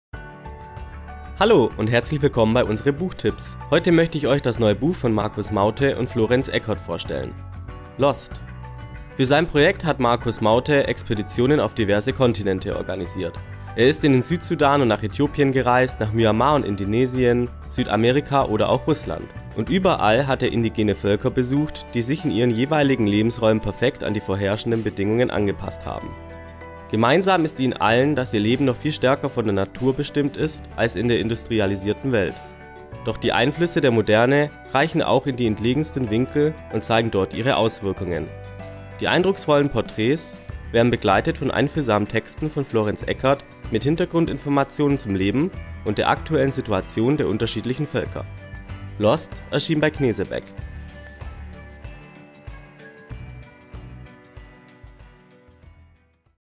Markus Mauthe/Florens Eckert | Lost | Roman Rezension | 80
Markus Mauthe beschreibt in seinem Buch seine Expeditionen auf verschiedenen Kontinenten der Welt. Dort hat er überall indigene Völker besucht, die sich in ihren jeweiligen Bedingungen angepasst haben.

Lost erschien bei Knesebeck.

Lost
Autor: Markus Mauthe und Florens Eckert
Seiten: 320
Preis: 50,00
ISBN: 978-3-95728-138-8

Lust bekommen auf mehr Buchtipps?